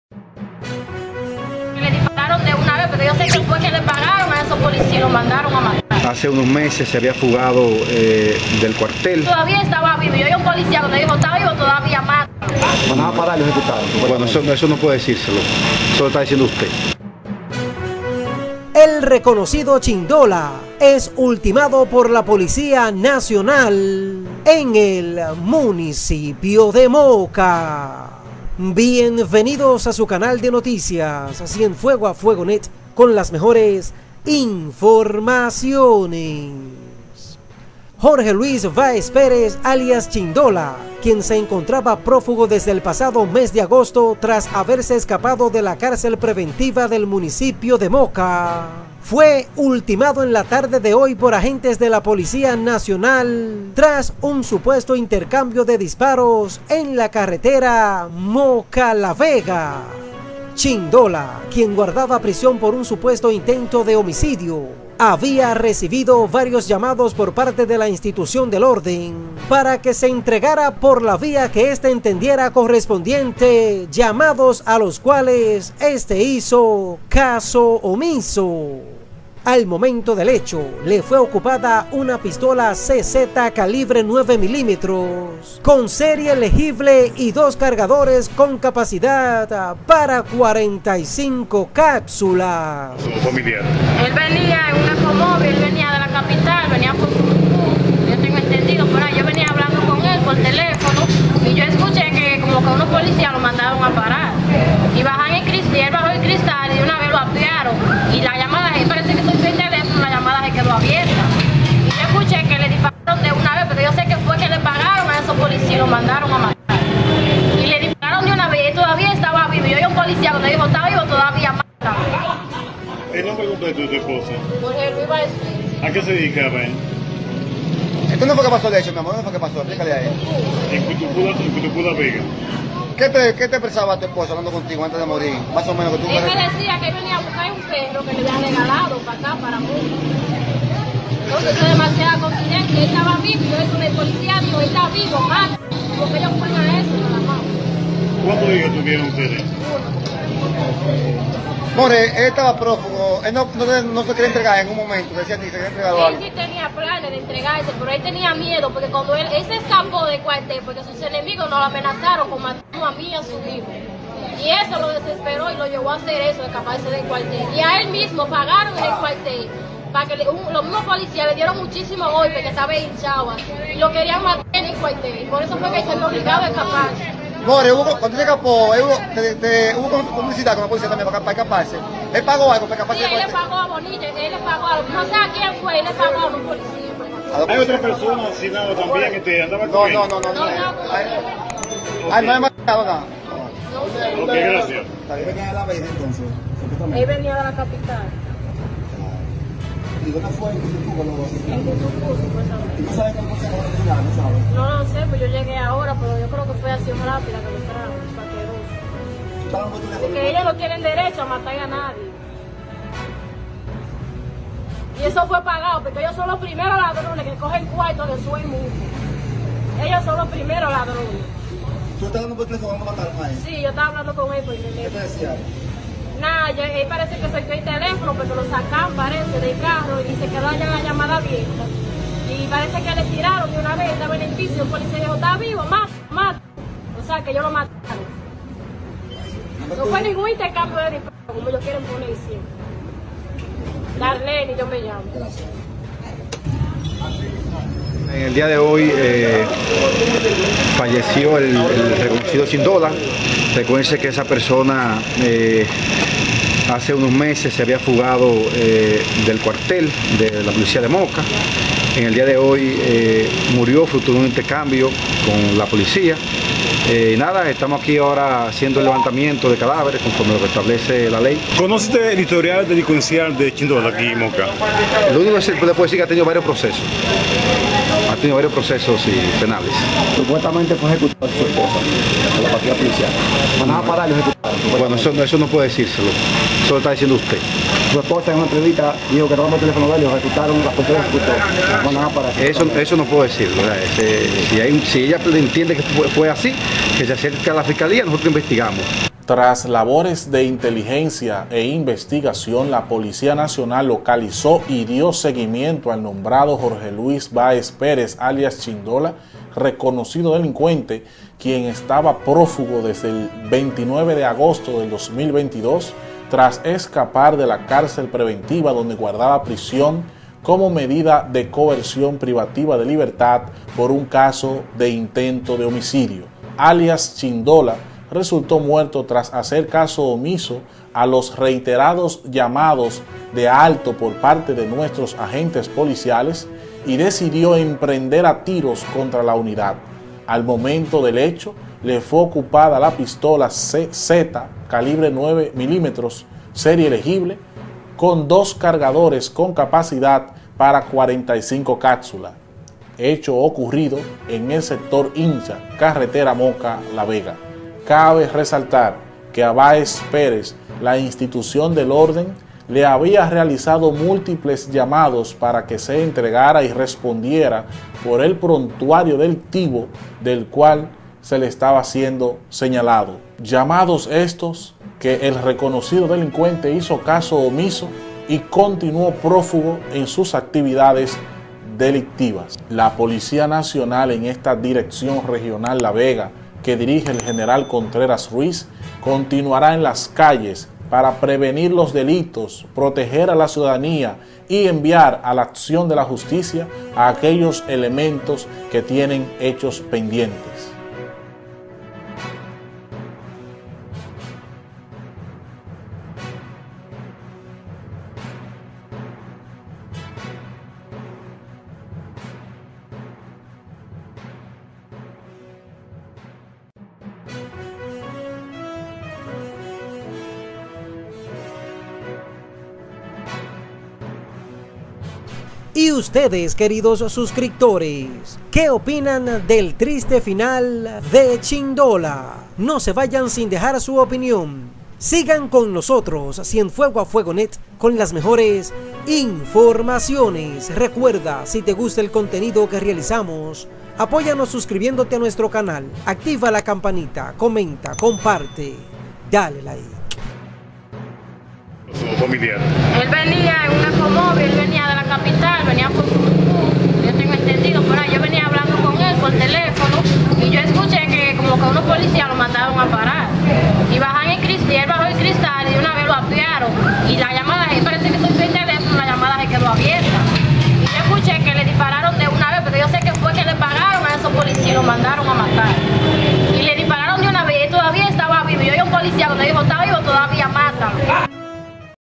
La Policia Se Lambe A Chindola En Moca
La Policia Se Lambe A Jorge Luis Baez Perez,  alias Chindola En Moca